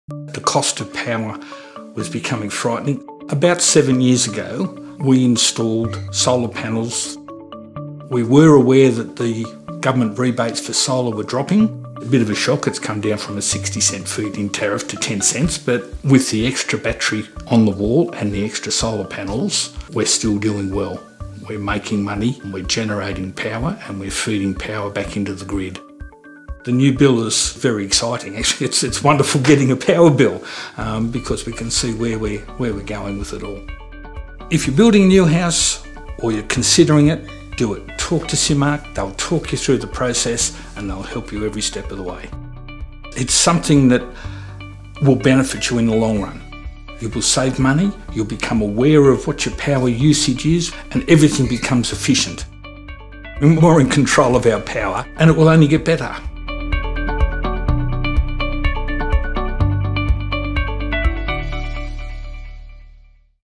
Tesla Powerwall in the Southern Highlands
Hear what one resident of Bowral, NSW is saying about his decision to invest in home energy storage through Simmark